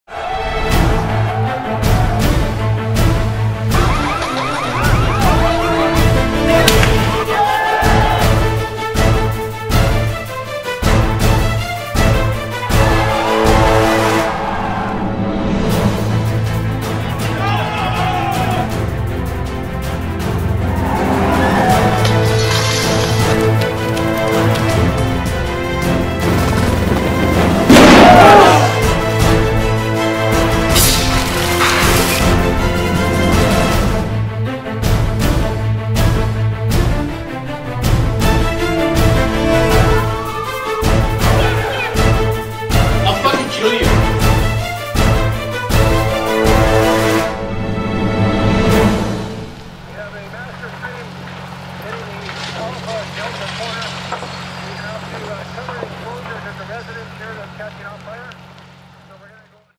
2013 Demo Reel: Best moments of Fire, Police & EMS
Breaking news and editorial 1080p video highlights all shot from 2010 to 2012.